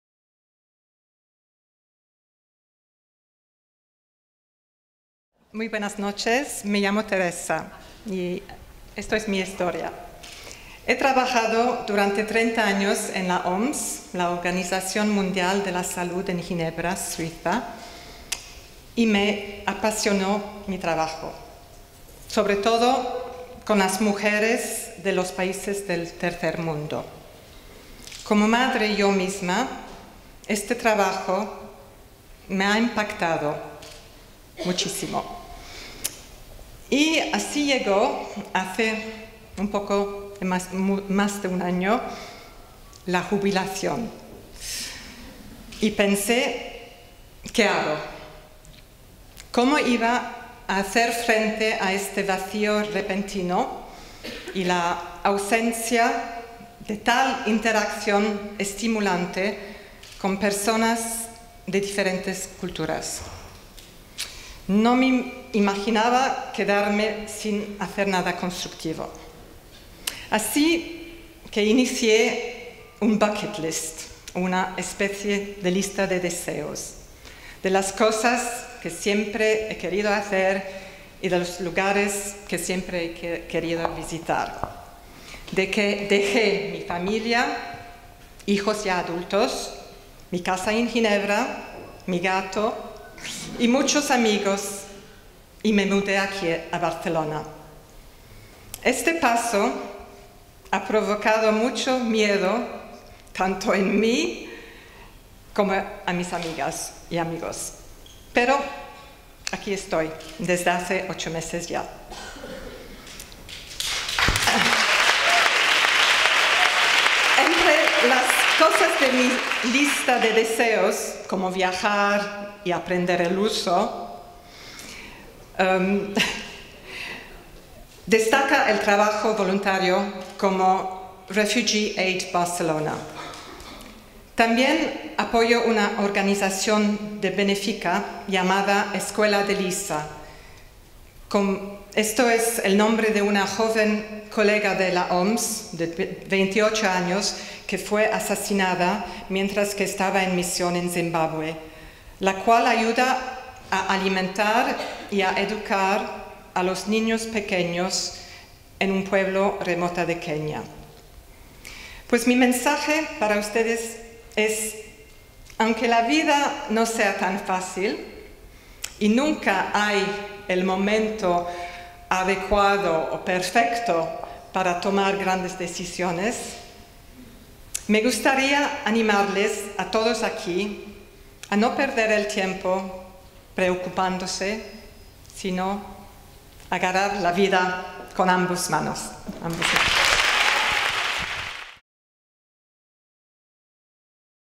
What would be on your Bucket List? I Teresa Harmand I Hub Dot
Good evening, my name is Teresa and this is my story
I worked for almost 30 years at the World Health Organization in Geneva, Switzerland and I was passionate about my job, above all working with women in the developing world.  As a mother myself, this work had a really big impact

And then just over a year a go now retirement came, and I thought what do I do? How would I deal with this sudden void and the absence of such stimulating interaction with people of different cultures? I couldn’t imagine a life not doing anything constructive

So I initiated a kind of “bucket list” of things I have always wanted to do and of places I have always wanted to visit.

I left my grown-up family, my house in Geneva, my cat and many friends and moved here to Barcelona. This move frightened me at first as well as many friends, but 8 months later, here I am!

On my bucket , as well as travelling and learning to (minute 2:00)  I´ve been working as a volunteer for Refugee Aid Barcelona

I also support a charity called Lisa’s School, named after a young WHO colleague who was murdered whilst on mission in Zimbabwe) that helps feed and educate very young children in remote villages of Kenya.

I have learnt that life isn’t easy and there is never a RIGHT moment to make a big decision but what I would encourage everyone to do is not to waste time worrying but to GRAB LIFE WITH BOTH HANDS!